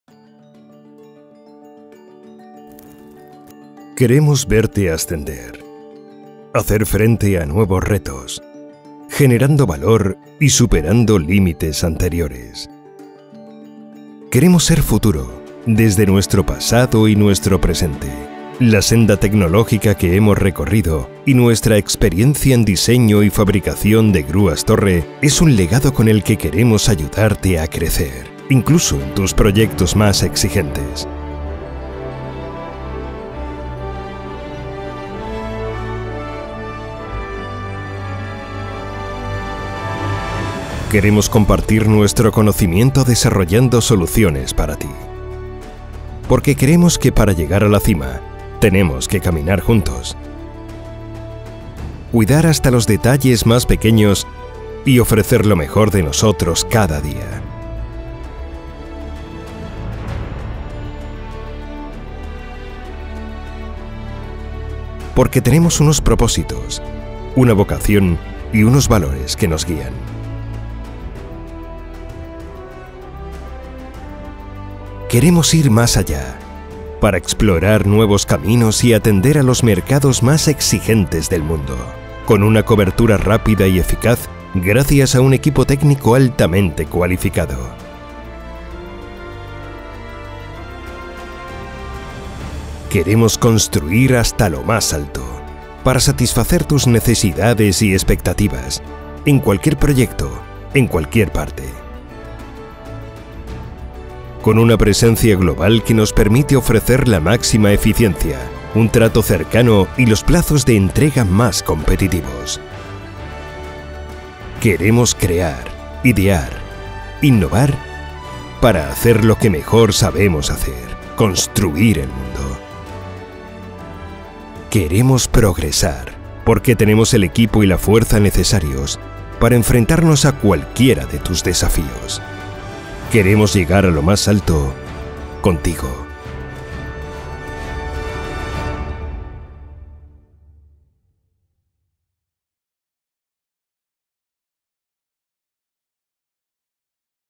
COMANSA - Vídeo Corporativo - Español
Vídeo corporativo del fabricante de grúas torre para la construcción COMANSA. Idioma español / castellano.

Committed To Lifting Evolution